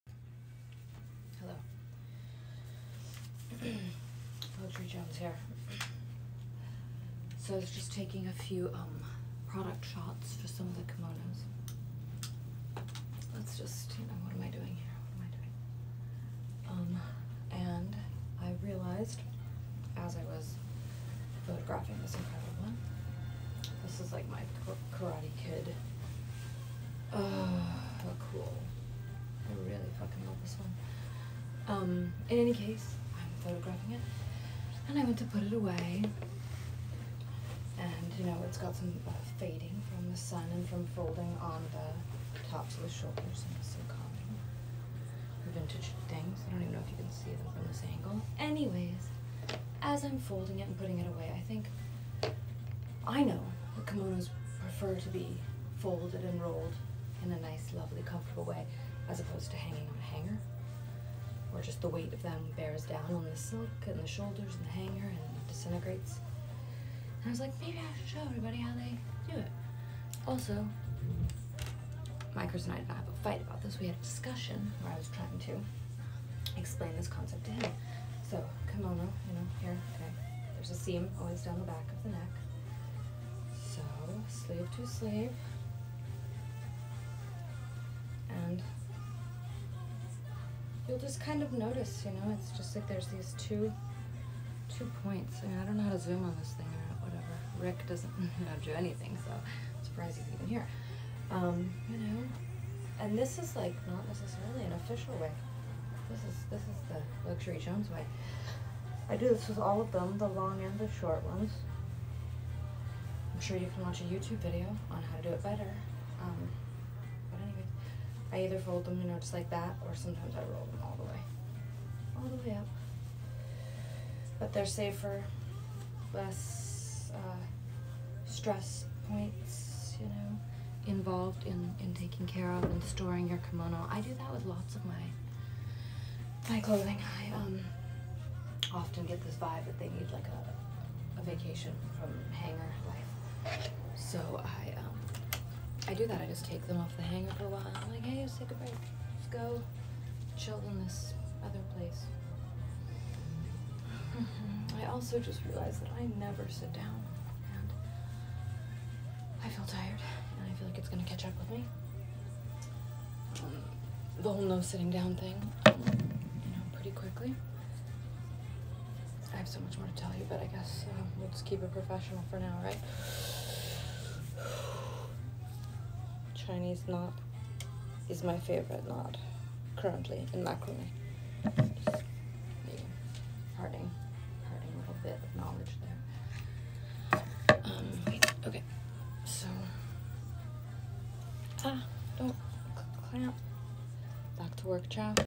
Let's Luxury Jones It: How to Fold a Kimono
"Luxury Jones". Though most often thought of as a noun (specifically a person or a clothing label) is actually a verb. It's a lifestyle, a way of doing things, a way of viewing the world. And I am here to show you how to Luxury Jones your life.
In this video I'll show you the Luxury Jones method for folding a kimono. Is this the official correct standard way to fold kimono? I have no idea, this is just how I felt the kimono wanted to be folded. it's just the way we do it over here.